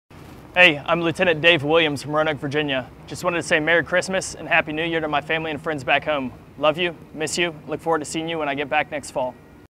1st Lt. David Williams
Credit: Lance Cpl. Kyle McNan | Date Taken: 12/15/2016
1st Lt. David Williams, public affairs officer with Special Purpose Marine Air-Ground Task Force-Crisis Response-Central Command, sends a holiday greeting to his family and friends back home in Roanoke, Virginia, while forward deployed in the Middle East.